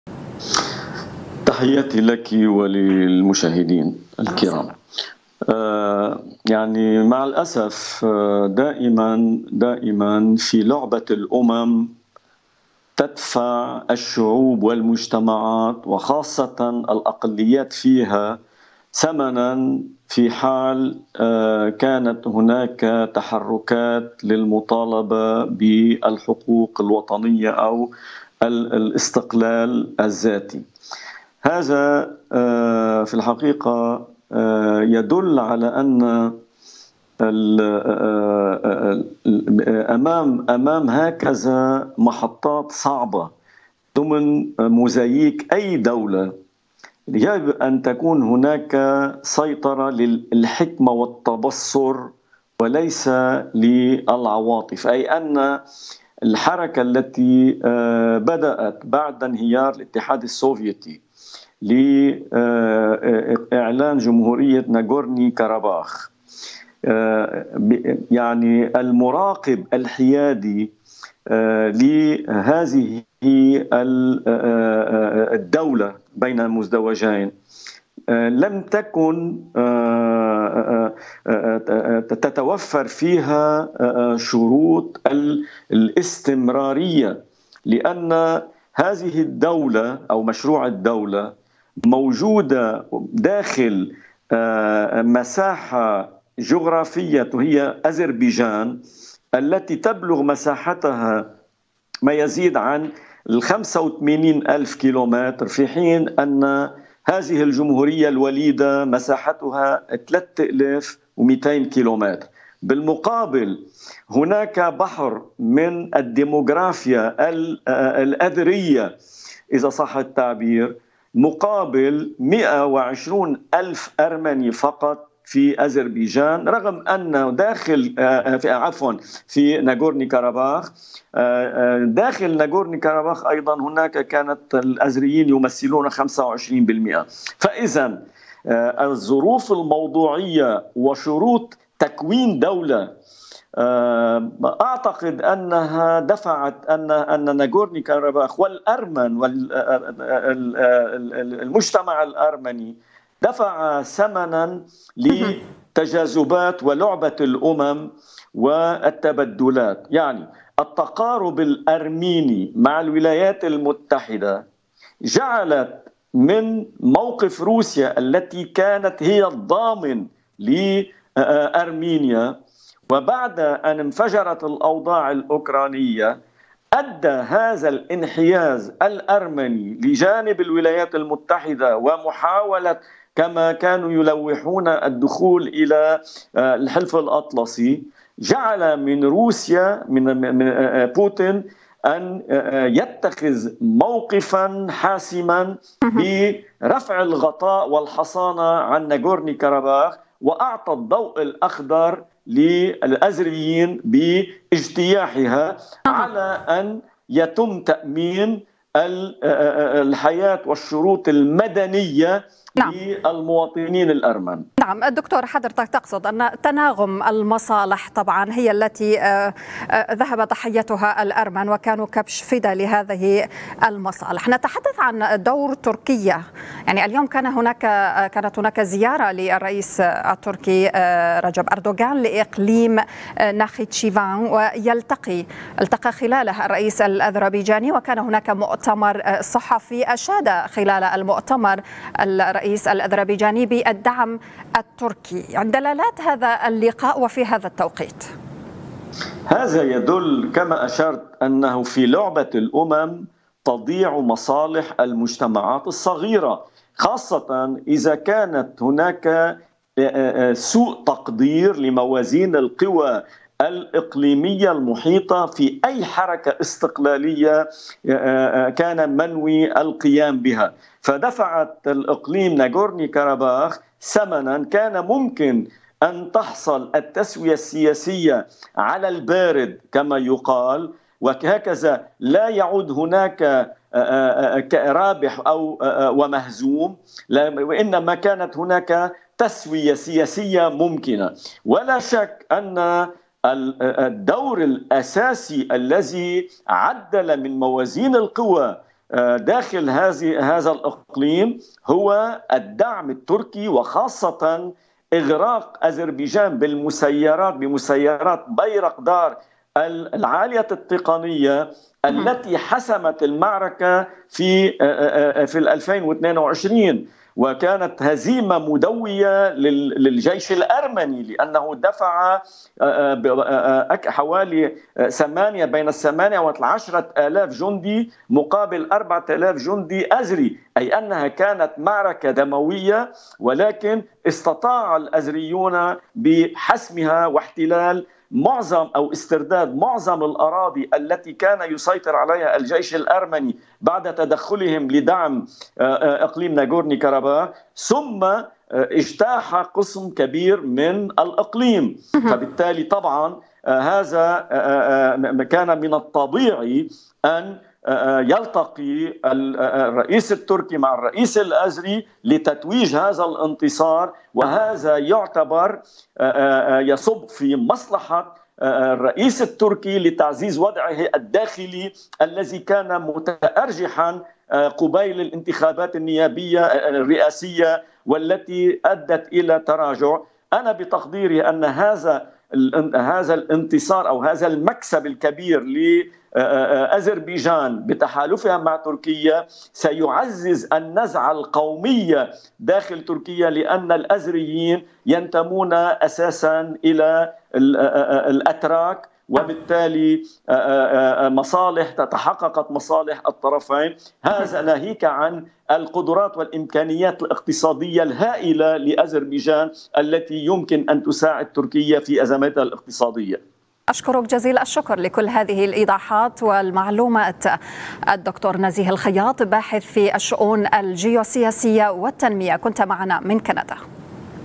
د. نزيه الخياط: دائماً في لعبة الأمم تدفع الشعوب والمجتمعات وخاصة الأقليات منها ثمناً في حال كانت ..
كندا - مونتريال
د. نزيه الخياط - باحث في الشؤون الجيوسياسية والتنمية

دائماً في لعبة الأمم تدفع الشعوب والمجتمعات وخاصة الأقليات منها ثمناً في حال كانت هناك تحركات للمطالبة بالحقوق الوطنية أو الاستقلال الذاتي.
البث الحي / البث المباشر الأعلى مشاهدة

تابعوا قناة اليوم على التردد:
12687_27500 (H) _NILESAT